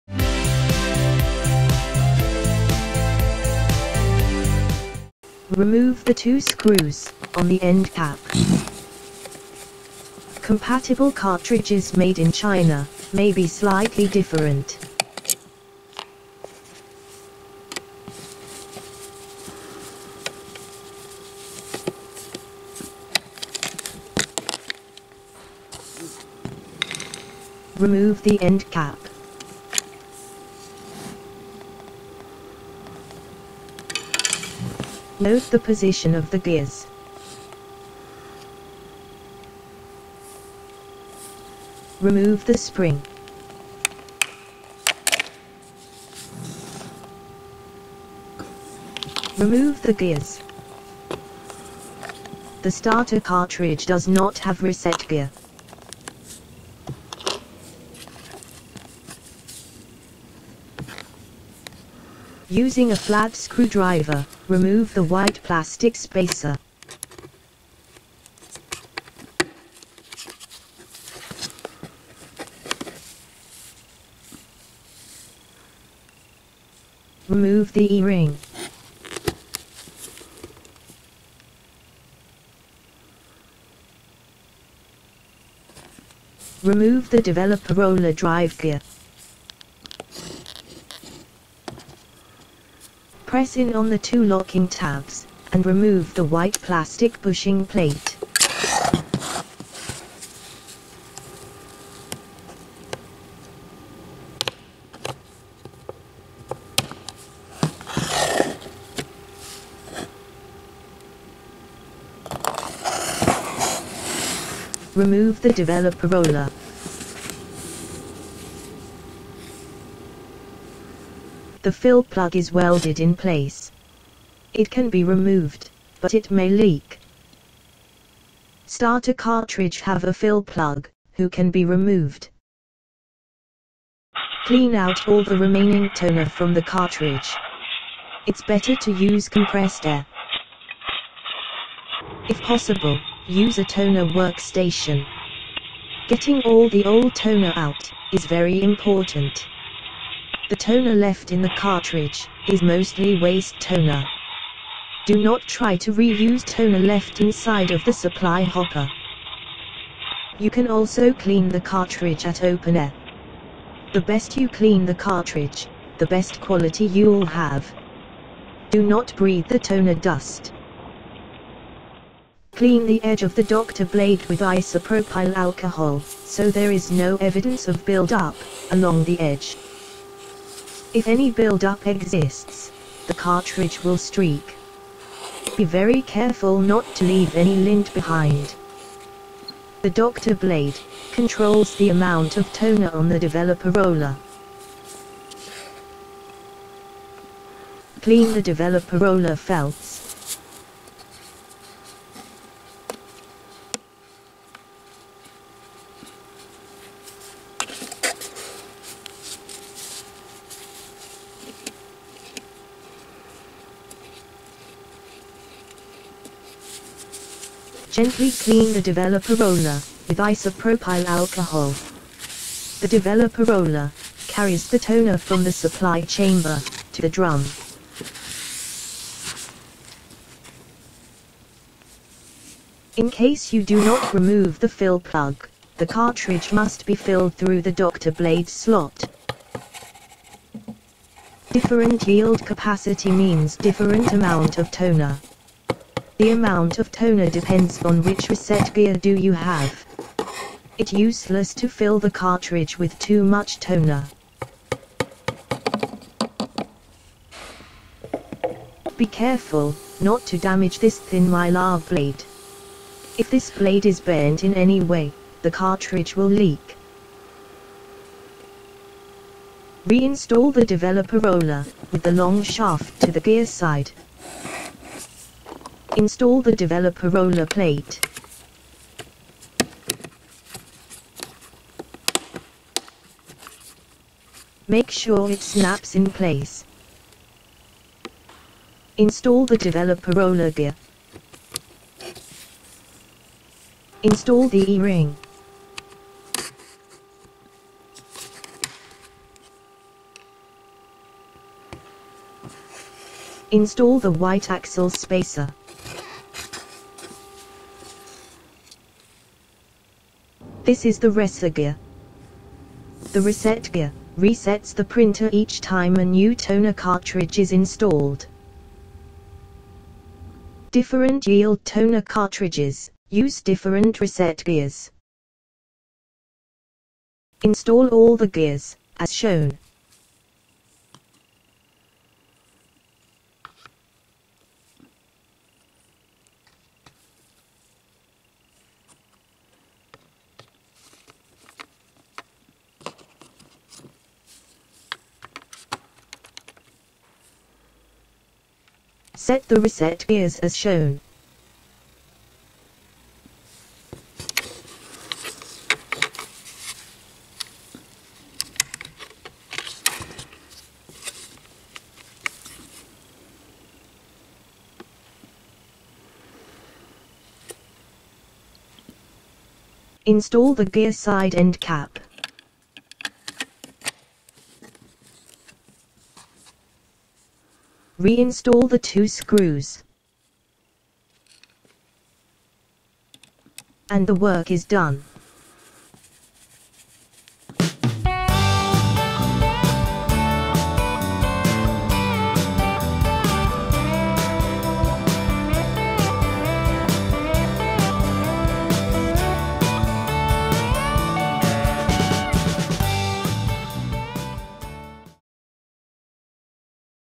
Brother MFC HL-L5000 TN880/3480 Toner Cartridge Refill Reset
How to refill and reset Brother toner cartridge TN880 TN3480 TN3430 TN-3520 for use in HL-L5000D HL-L5100DN HL-L5200DW HL-L5200DWT HL-L6200DW HL-L6200DWT HL-L6250DW HL-L6300DW HL-L6400DW HL-L6400DWT DCP-L5500DN DCP-L5600DN DCP-L5650DN MFC-L5700 MFC-L6700DW MFC-L6750DW MFC-L6800DW MFC-L6900DW MFC-8950DW MFC-8950DWT L5800 LDW5850

There are four different toner cartridges:
TN820/TN3430/TN3420 3,000 pages,
TN850/TN3480/TN3440 8,000 pages
TN880/TN3512/TN3470 12,000 pages.
TN890/TN3520/TN3490 20,000 pages but only fit into HL-6400 / MFC-L6900 series.

The part number for the toner cartridges depend on the region.
America TN820 TN850 TN880 TN890
Europe TN3430 TN3480 TN3512 TN3520
Asia/Oceania TN3420 TN3440 TN3470 TN3490

Reset gear reset the printer and tell the printer if there is a standard yield, high yield, super high yield or ultra high yield cartridge installed.
When you install a new toner cartridge, the bias voltage is set to a high. As the toner cartridge is used, the bias voltage is reduced gradually down. This is necessary because a new toner cartridge has a tendency to print light.

Things needed:
- Toner
- Screwdriver
- Isopropyl alcohol